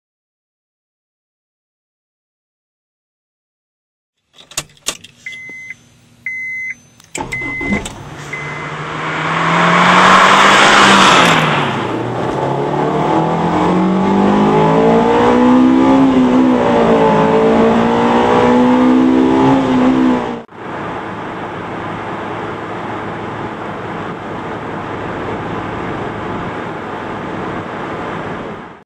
PLX M-300 Wideband O2 in S2000
PLX Devices' best selling wideband oxygen sensor controller installed in a honda S2000 doing partial throttle and wide open throttle runs.